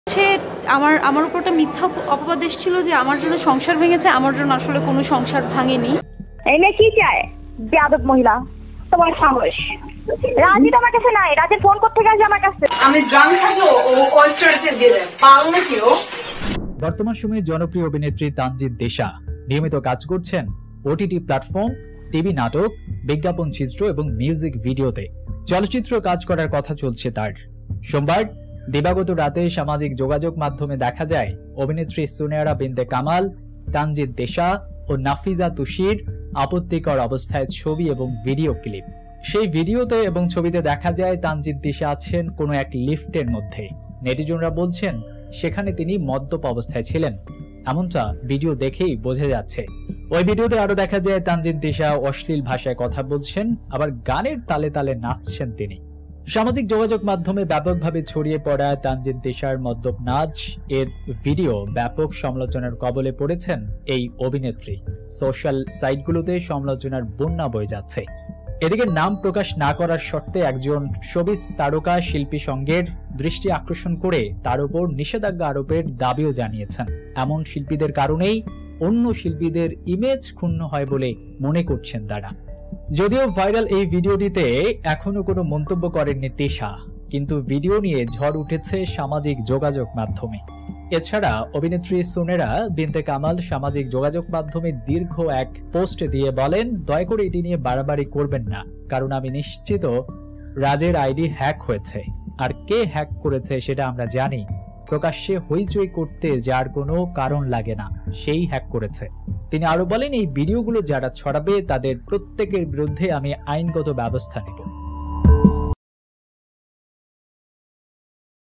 অন্তরঙ্গ ভিডিও তোপের মুখে তানজিন তিশা ! Sariful razz | Tanjin tisha | viral video | breaking news
শরিফুল রাজের সাথে অন্তরঙ্গ ভিডিও ভাইরাল তোপের মুখে তানজিন তিশার ! Sariful razz | Tanjin tisha | viral video | breaking news